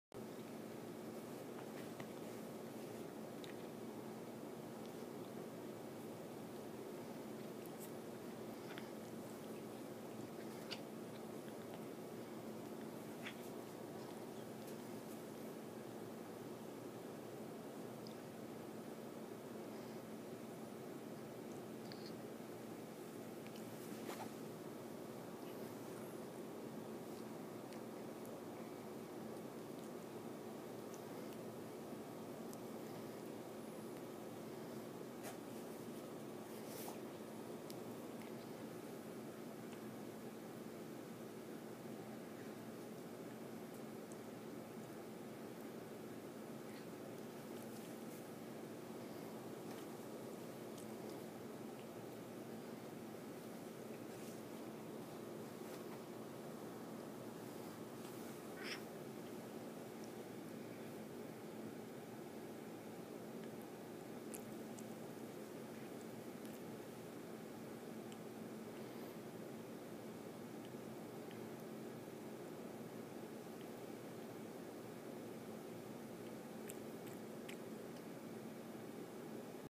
NomNom Serif
Serif messing with my hand.